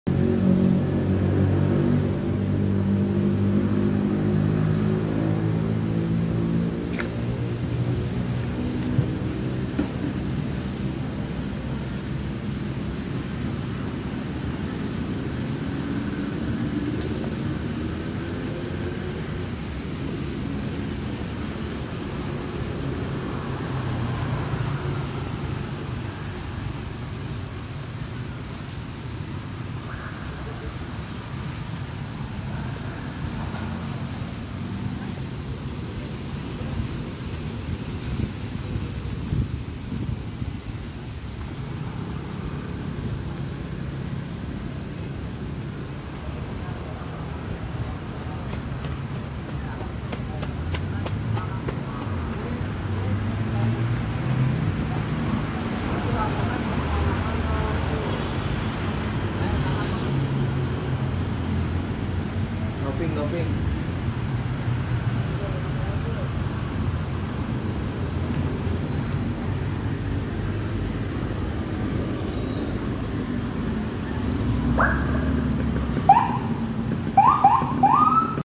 Taman Suropati
Taman Suropati or Suropati Park is a public park located in Menteng, Jakarta, Indonesia. The park is located in the center of Menteng and is the meeting point of three streets: Teuku Umar Street, Diponegoro Street, and Imam Bonjol Street.